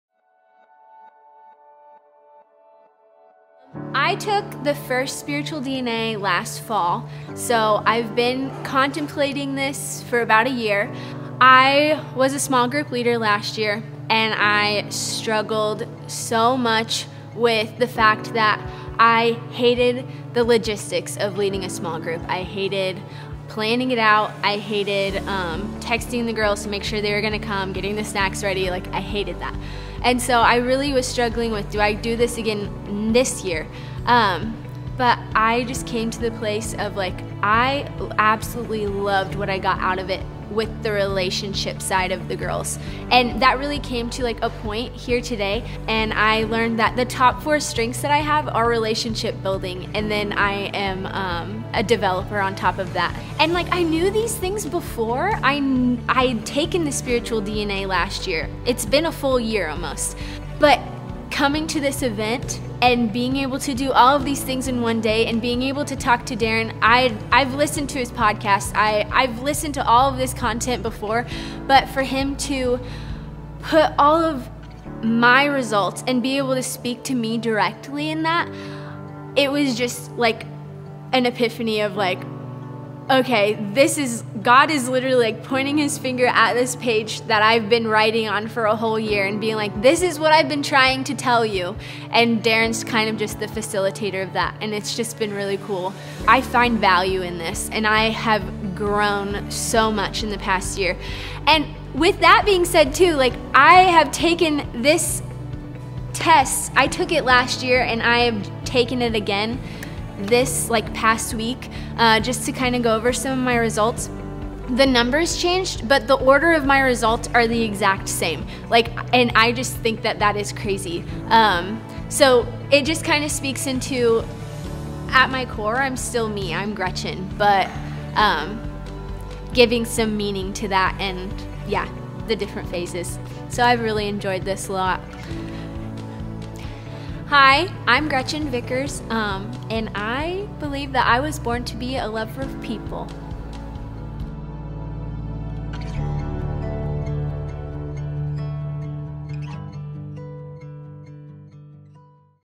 Gretchen shares her Spiritual DNA story
Gretchen shares a little of her story, how she has been uncovering her Spiritual DNA over the past year, and her experience at our recent Spiritual DNA live event. Our next Spiritual DNA Live event is on February 9, 2019 at Mercy Road Church in Carmel, Indiana.
Register at spiritualdna.me